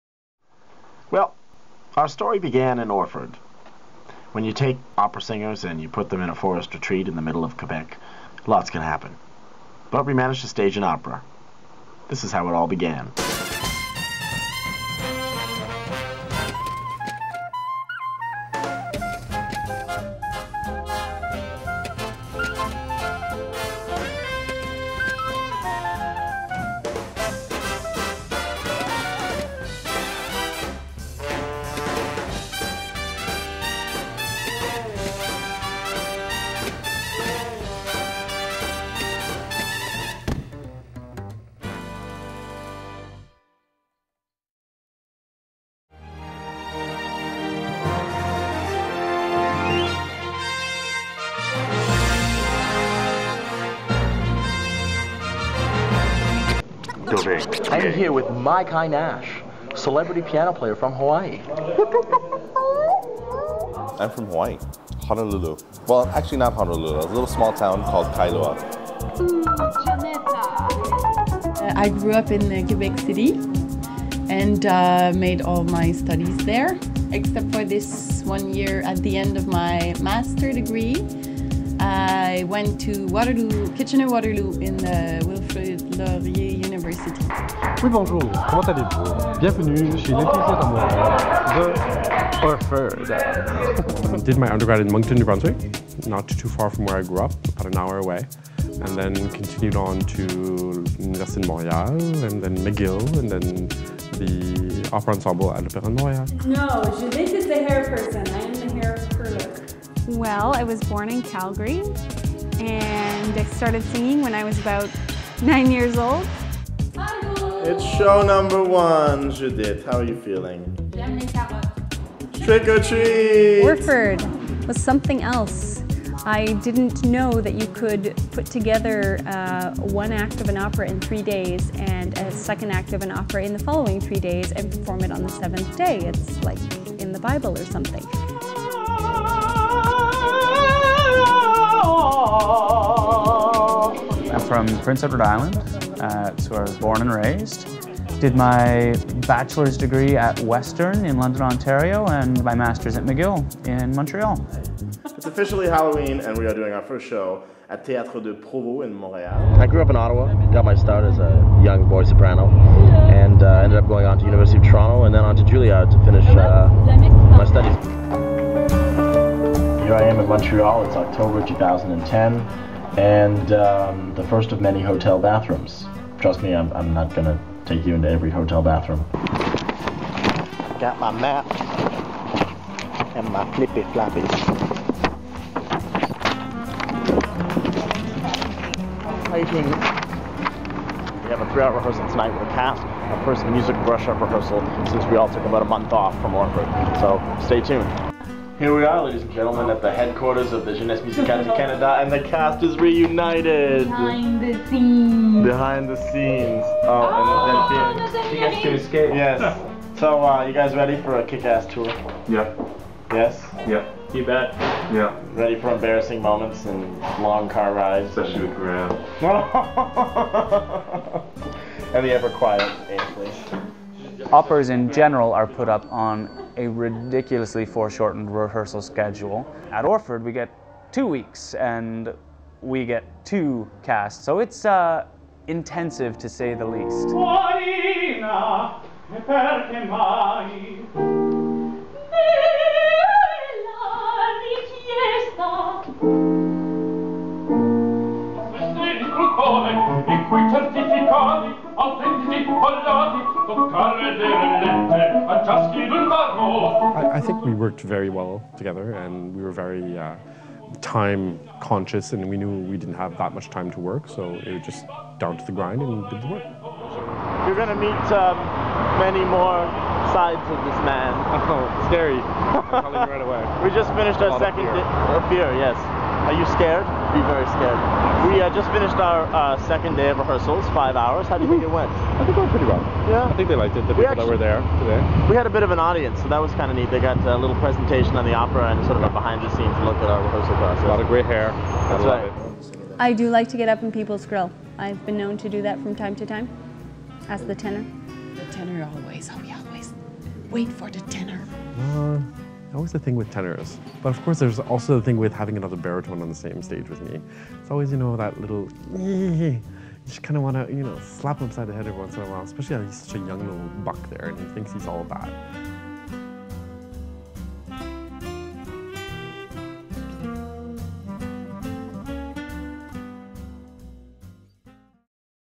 A Singer's Life 101: Montreal
Meet the cast as we reunite in Montreal to prepare for the road, having staged the opera 6 weeks earlier in Orford, Quebec. Drama follows Diva's wherever they go.